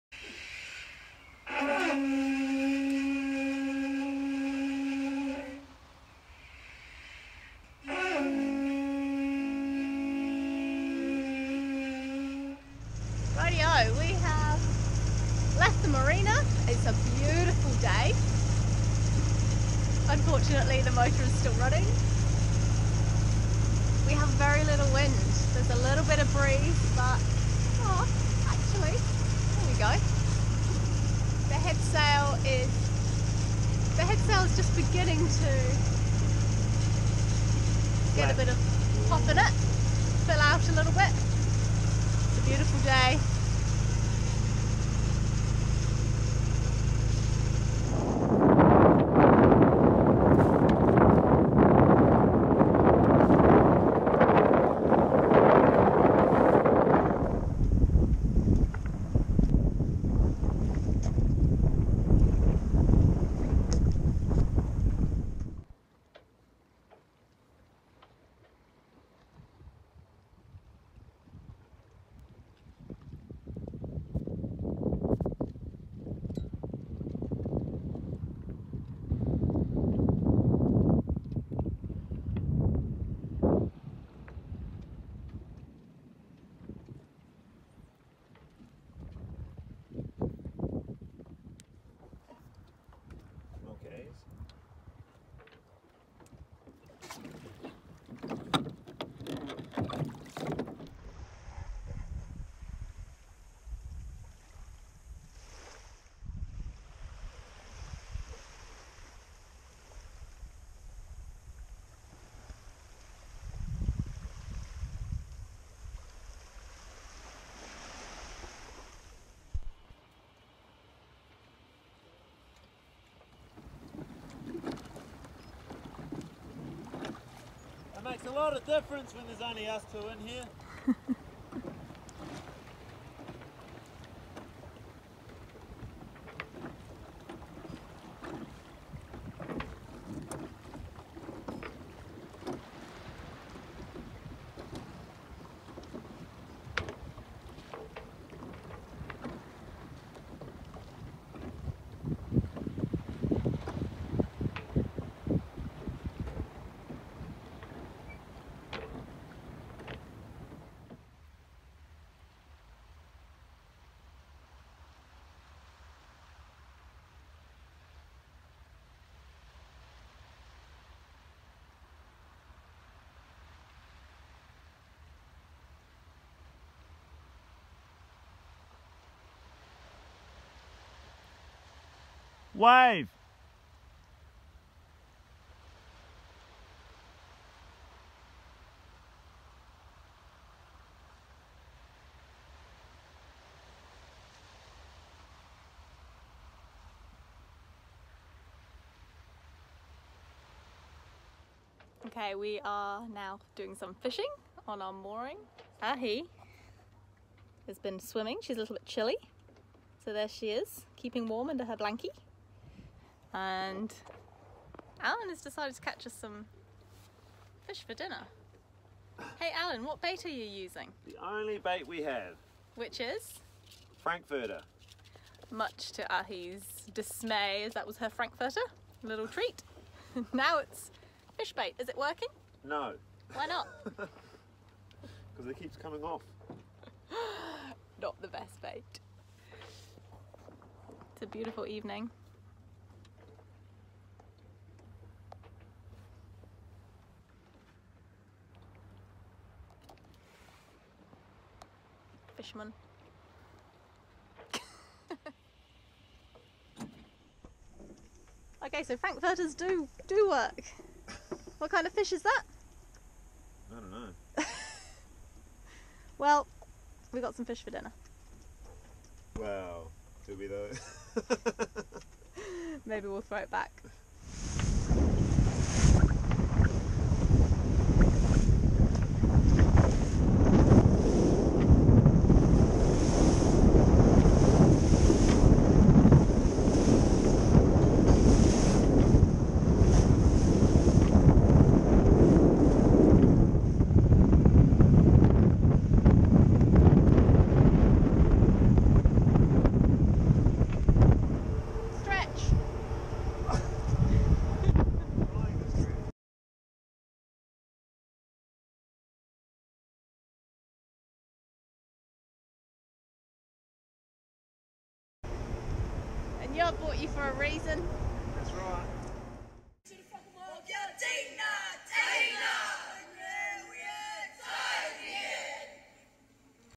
Overnighter in L.P.C. Aotearoa with crew Al T & Ahi! Sailing my Contessa 32, Nerissa K.
A two day trip to Little Port Cooper - rowing to shore, fishing and a decent enough headwind home. Just testing some of the new work I have done on my boat.